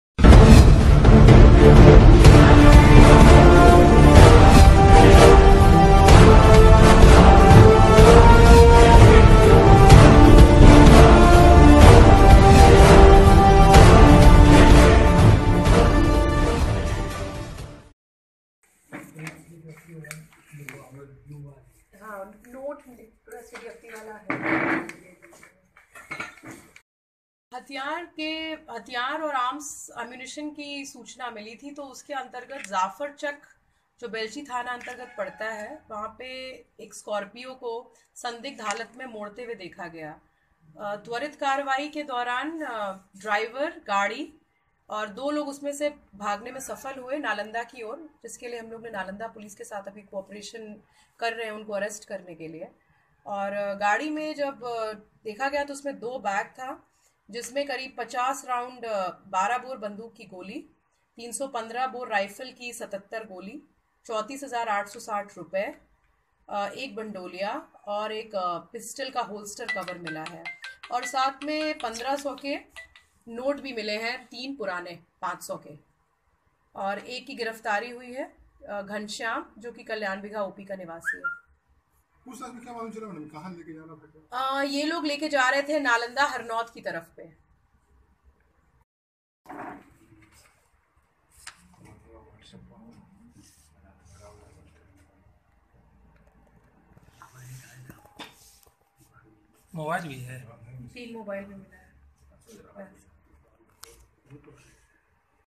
एएसपी लिपि सिंह ने हथियार तस्कर को दबोचा!
बाढ़(न्यूज क्राइम 24): बाढ़ के एएसपी लिपि सिंह ने गुप्त सूचना के आधार पर हथियार तस्कर को दबोच लिया। जो गोली की बड़ी खेप लेकर नालंदा जिला जा रहे थे ,इसी क्रम में बेलछी थाना क्षेत्र के जाफर चक गांव के पास घात लगाकर स्कॉर्पियो के साथ एक हथियार तस्कर को गिरफ्तार कर लिया गया। हालांकि इस कार्रवाई में तीन व्यक्ति गाड़ी छोड़कर भागने में सफल रहे। वहीं एक पुलिस की गिरफ्त में आ गया। पकड़े गए अपराधी का नाम घनश्याम यादव है जो नालंदा जिले के हरनौत का रहने वाला बताया गया है , उसके पास से 315 बोर का 77 जिंदा कारतूस 12 बोर का 50 जिंदा कारतूस, तीन मोबाइल और ₹34860 नगद बरामद किए गए हैं। पुलिस एक और संदिग्ध व्यक्ति को हिरासत में लेकर पूछताछ कर रही है लेकिन उसके पास लाइसेंसी राइफल होने के चलते अभी और भी अनुसंधान जारी है, वहीं अनुमंडल पुलिस पदाधिकारी ने बताया कि इस का मास्टरमाइंड रुदल यादव नालंदा जिला का शराब तस्कर है। वह भी इस कांड में भागने में सफल रहा है ,जिसकी गिरफ्तारी के लिए पुलिस प्रयास कर रही है। साथ ही पुलिस अब गिरफ्तार अपराधी से प्राप्त जानकारी के आधार पर हथियार और गोलाबारूद की तस्करी के पूरे रैकेट को उखाड़ फेंकने के लिये दृढ़ संकल्पित हो गई है।